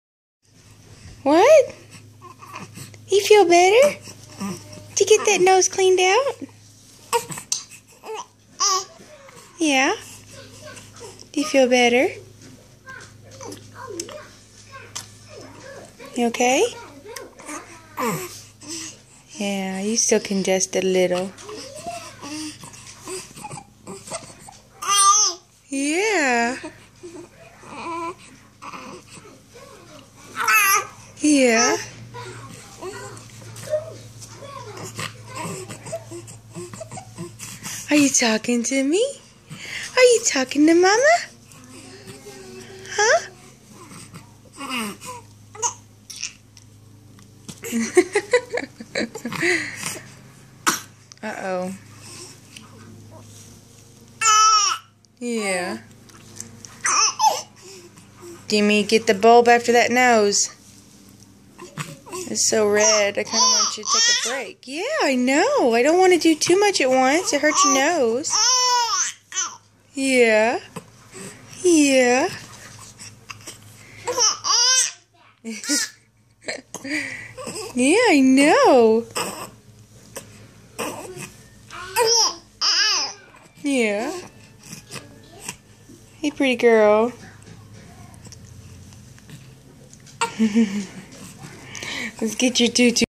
Her Way Of Telling Me... Part 1
Her Way of telling me she hates the nose bulb and being congested. .